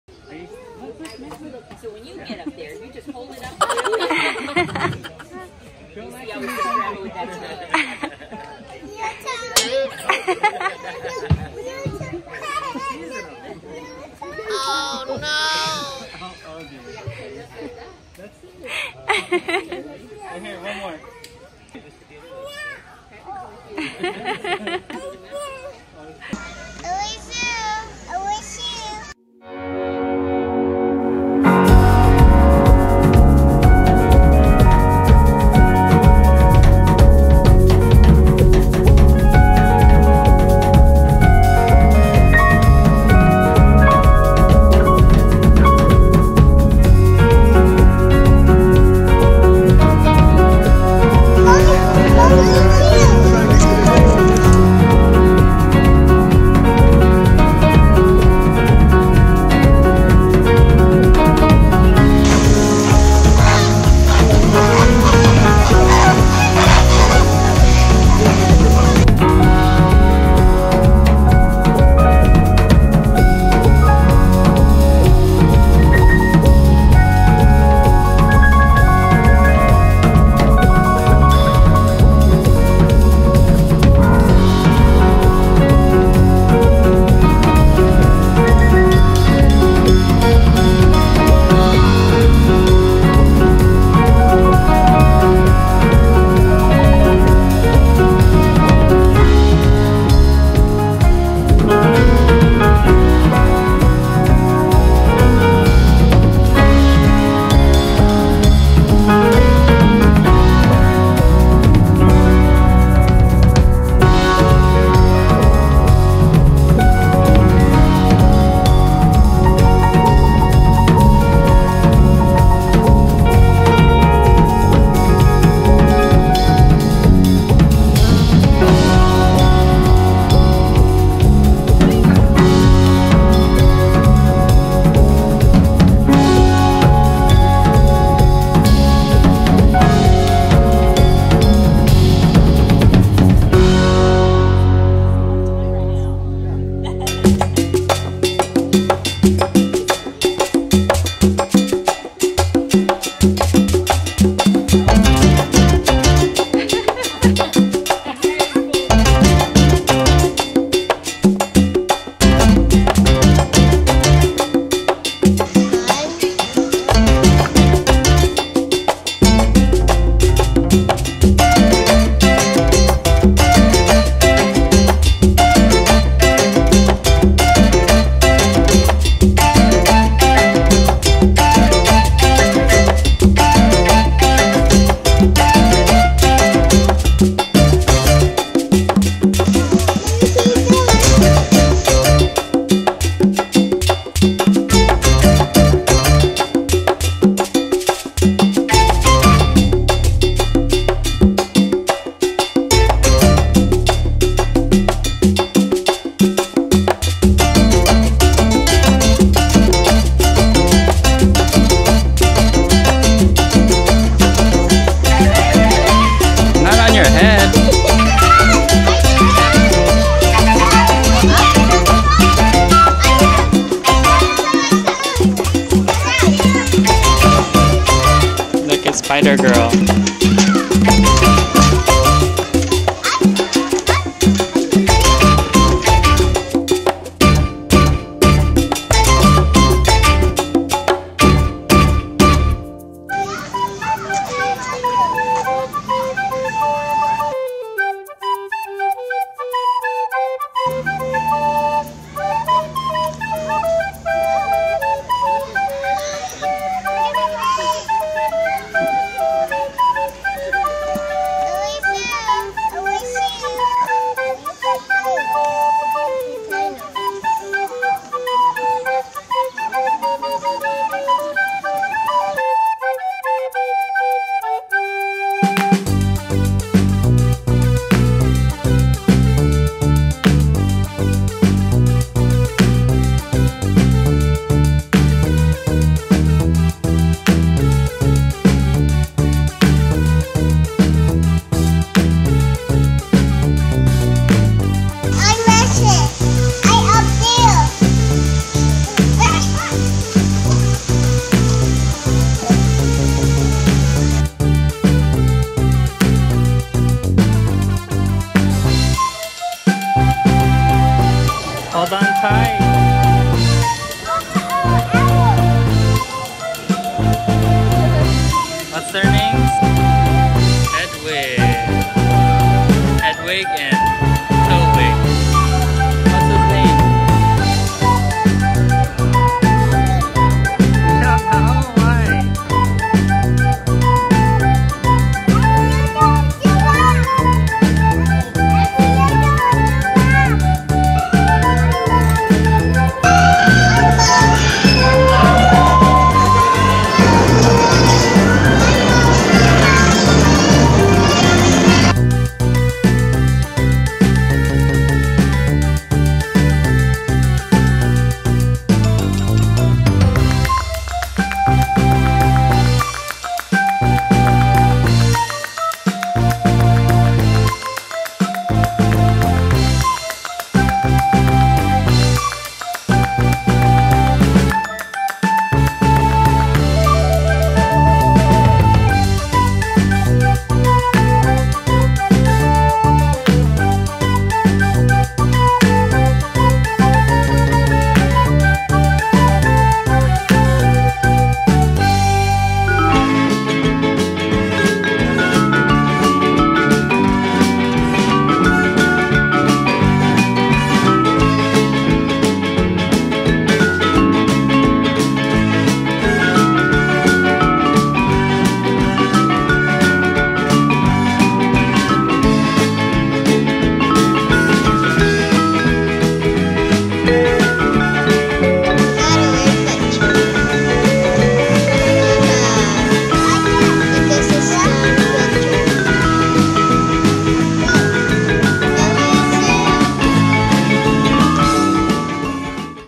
Amazing, Cool, and Cute Zoo Animals - LOS ANGELES ZOO
The Yutopians visit the LA Zoo for the first time and see a bunch of animals we never have seen before.